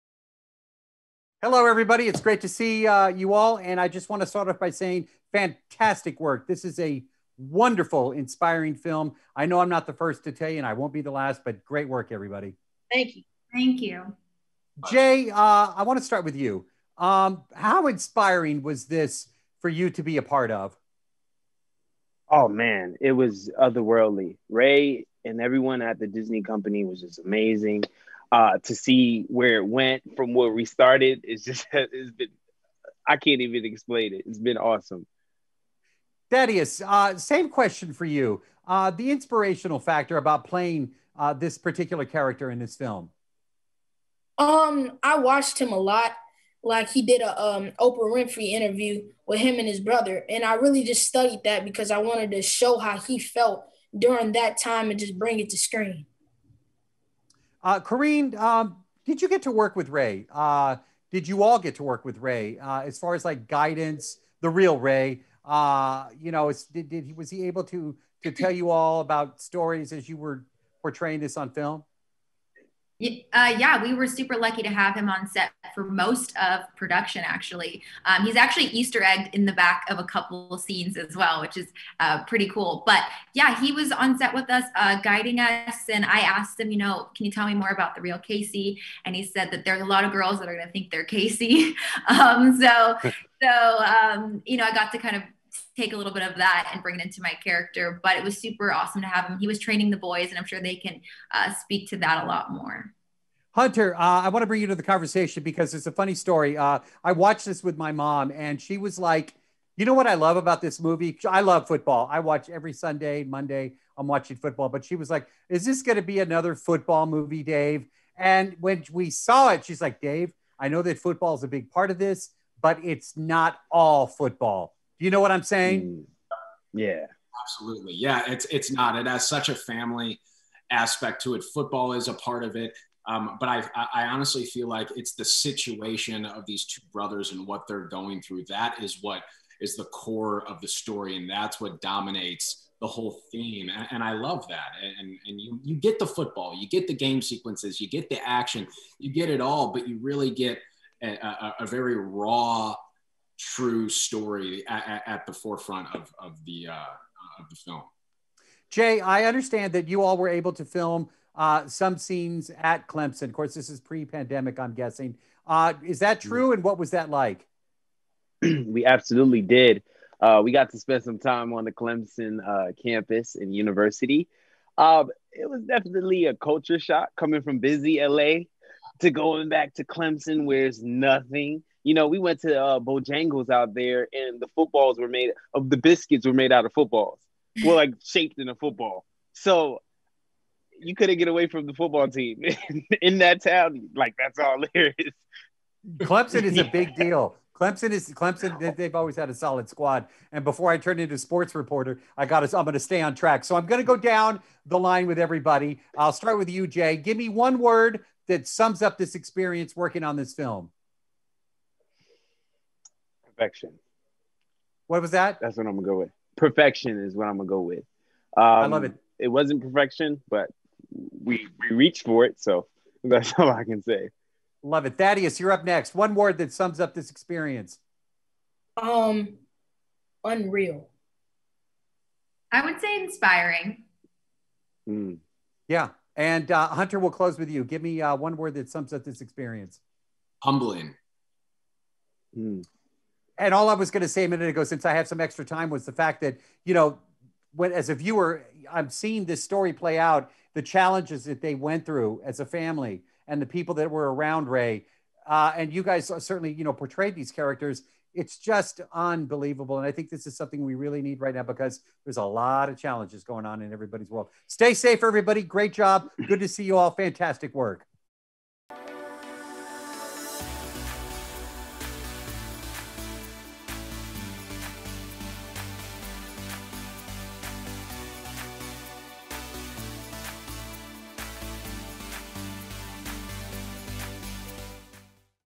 SAFETY: Backstage with Jay Reeves, Thaddeus Mixson, Corinne Foxx & Hunter Sansone
The stars of Disney+ SAFETY join Dave Morales for a backstage look at the movie based on the true story of Clemson University football player Ray McElratheby.

Joining us in the interview are stars Jay Reeves, Thaddeus Mixson, Corinne Foxx and Hunter Sansone.

SAFETY is now streaming on Disney+.